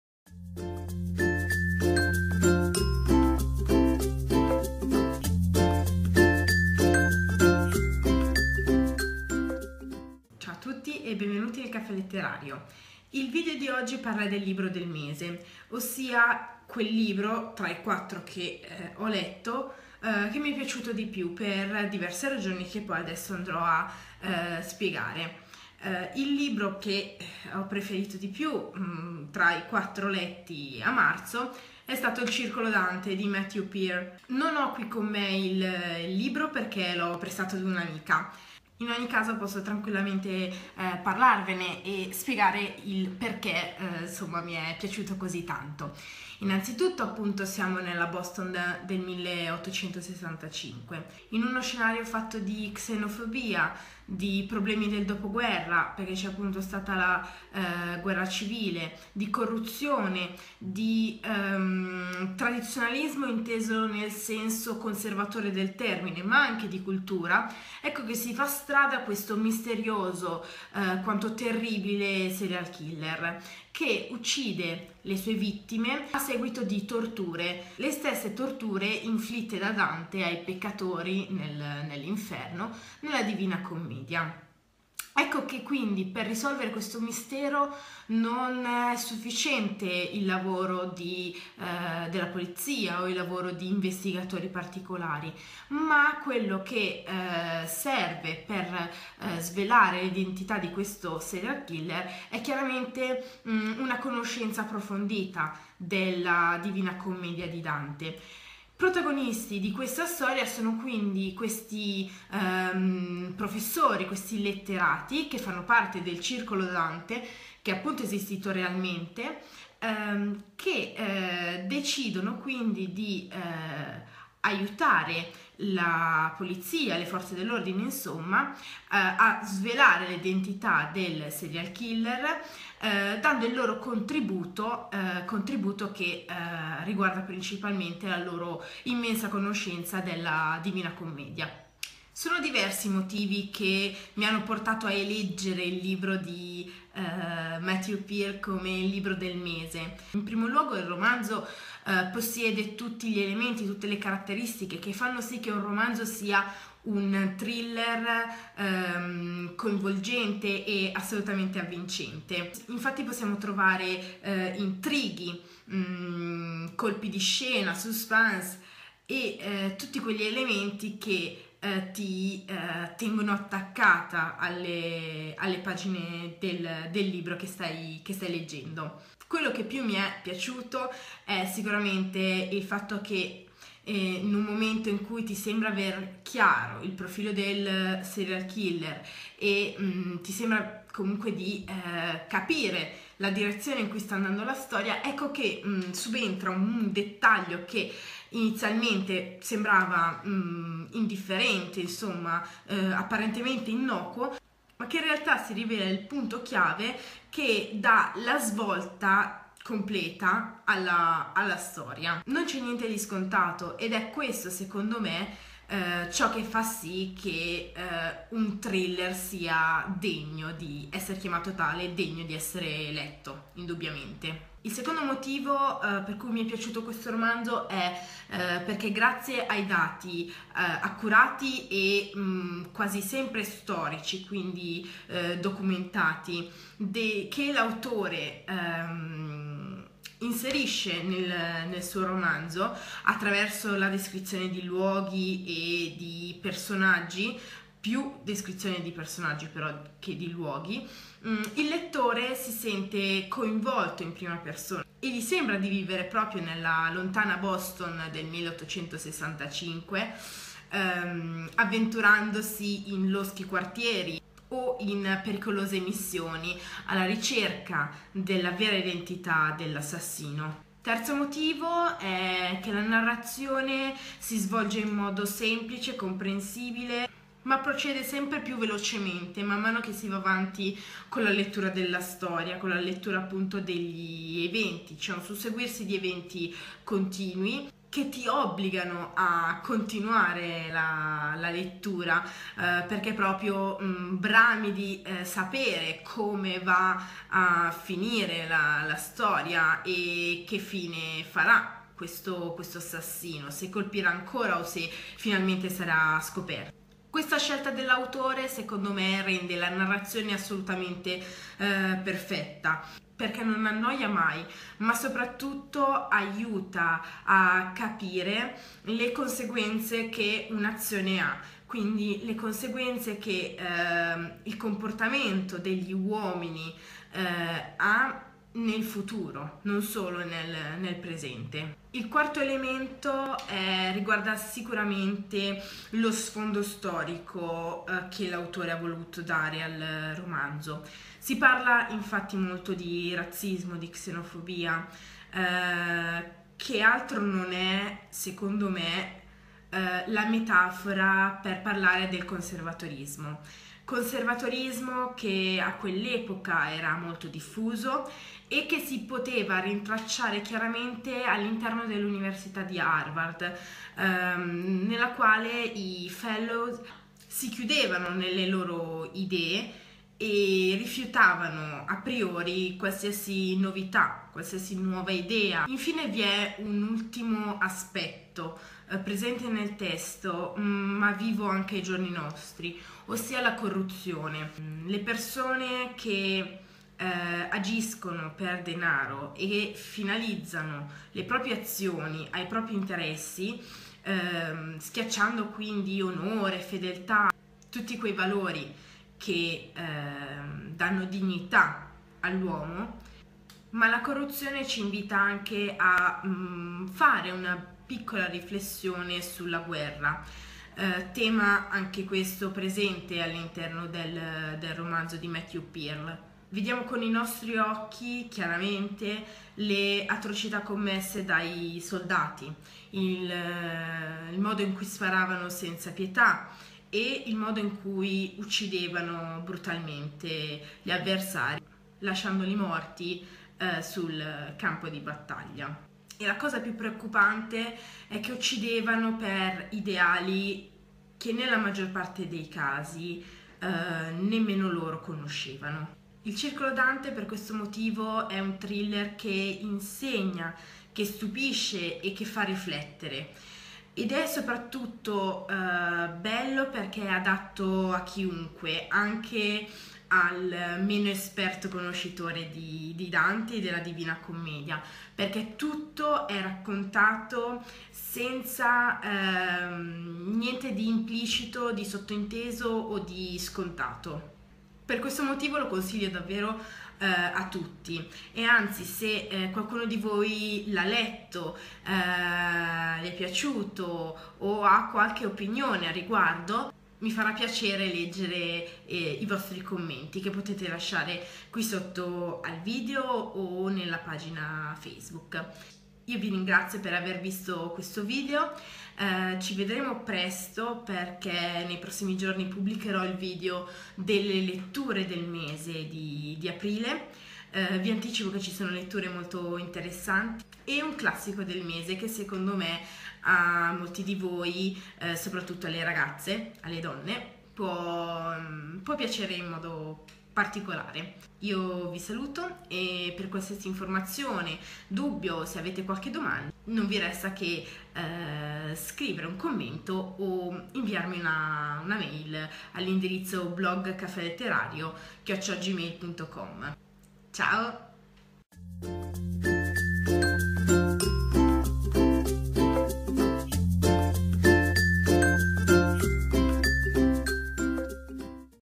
Il libro del mese: Il Circolo Dante - Matthew Pearl
Ciao a tutti lettori e ben ritrovati in un nuovo video.

Oggi parliamo del libro del mese, quel libro che, tra le quattro letture, è stato quello che più mi è piaciuto.

Il libro del mese di marzo è: "Il circolo Dante" di Matthew Pearl